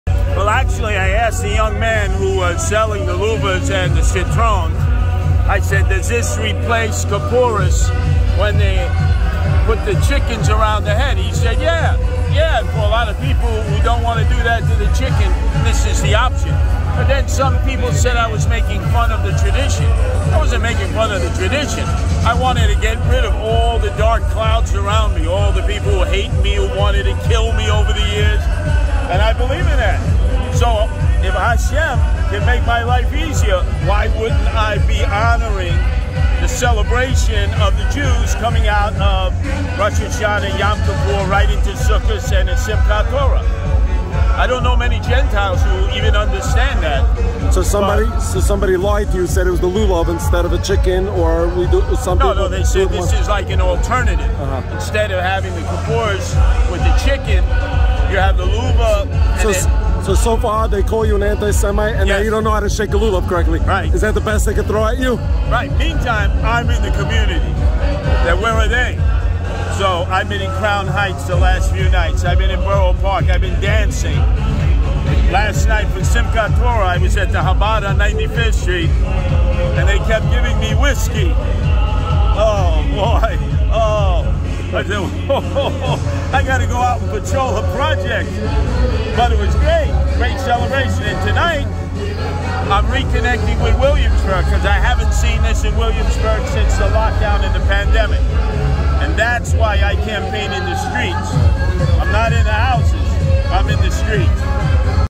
Curtis Sliwa Explains The Lulav Controversy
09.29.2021
Brooklyn, NY

Curtis Sliwa was accused by a Jewish new site of making fun of Jewish traditions with how he shook a lulav.  But what was the intent?  As explained, it is obvious Sliwa was set up by Adams supporters who lied to him on what to do with a lulav and then ran with a video saying he is making fun of Jews.